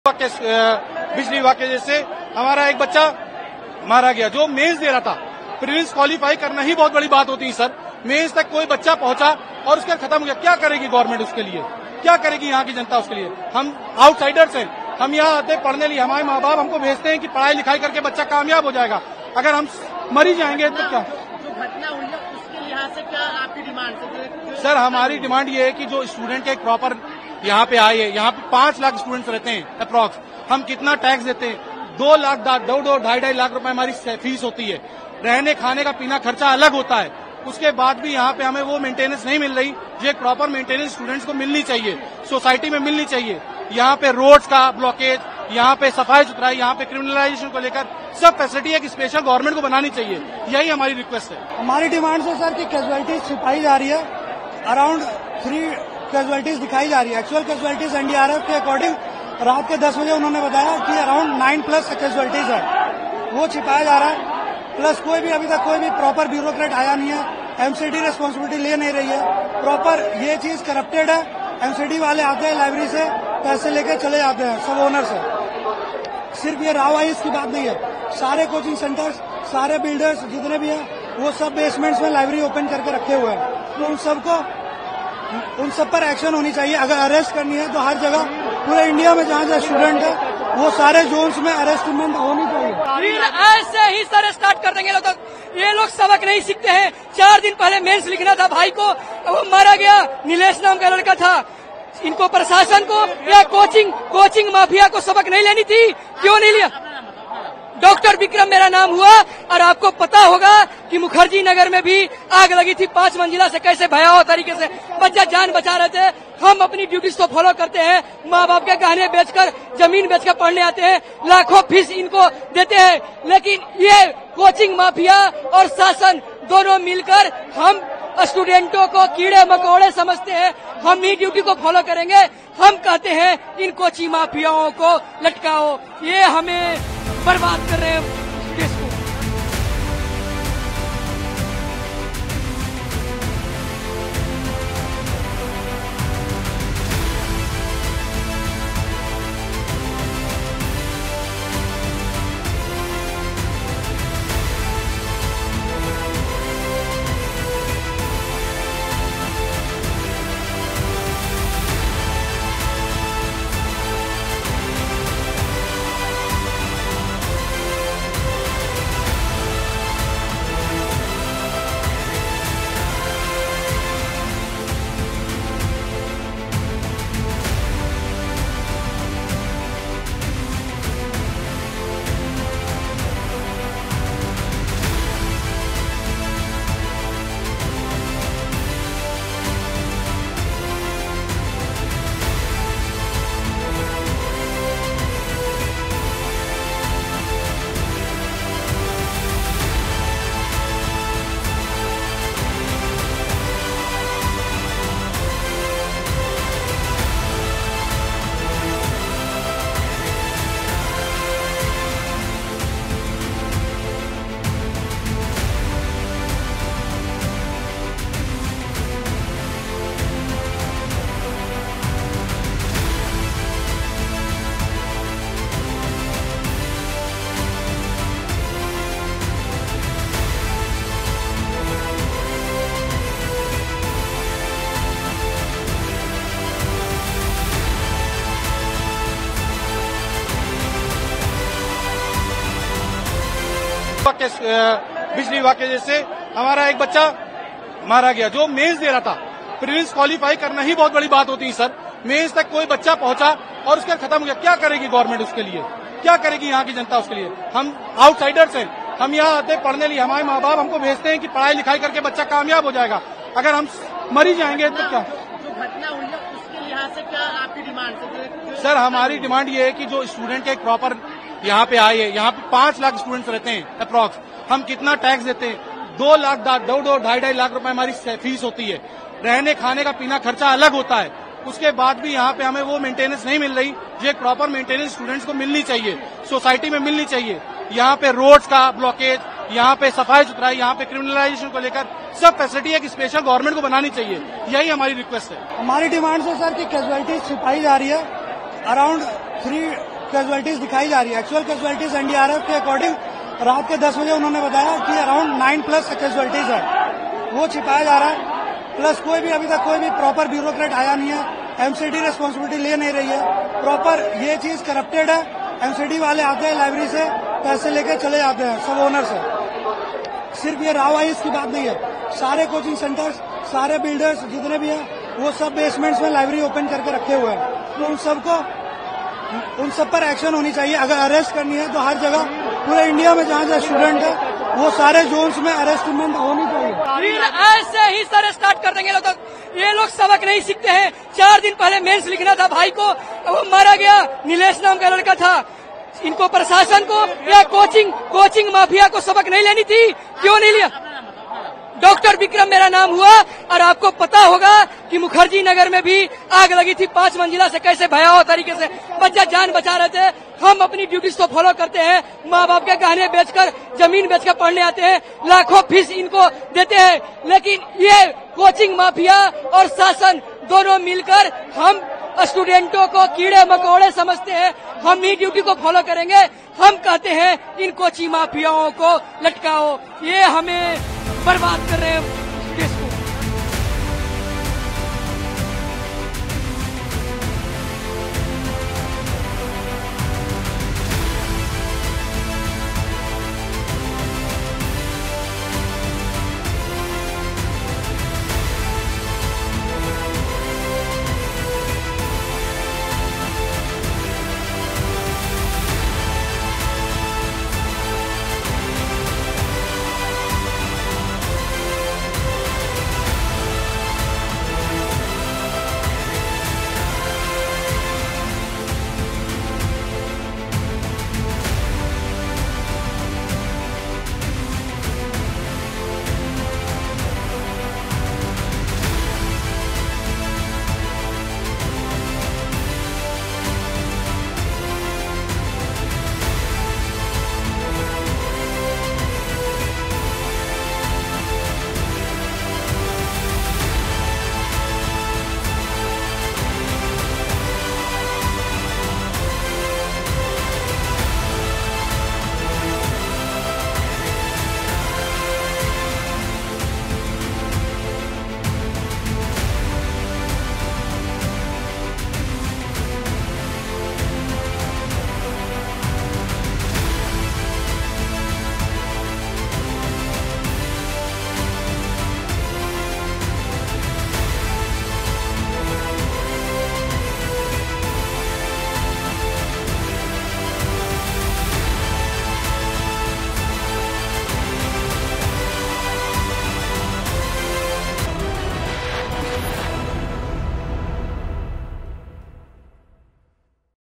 Old Rajendra Nagar Coaching Incident: तीन छात्रों की मौत, Delhi में छात्रों का विरोध प्रदर्शन
राजधानी दिल्ली के राजेंद्र नगर में स्थित एक आईएएस कोचिंग सेंटर के बेसमेंट में बारिश का पानी भर गया, जिसमें डूबने से 3 छात्राओं की मौत हो गई। शनिवार रात को हुए इस हादसे के बाद एनडीआरएफ की टीम मौके पर पहुंची और बचाव कार्य शुरू किया। दिल्ली सरकार ने मामले की न्यायिक जांच के आदेश दिए हैं। एएनआई के अनुसार रविवार सुबह भी बेसमेंट में पानी भरने के बाद बचाव और राहत कार्य जारी है।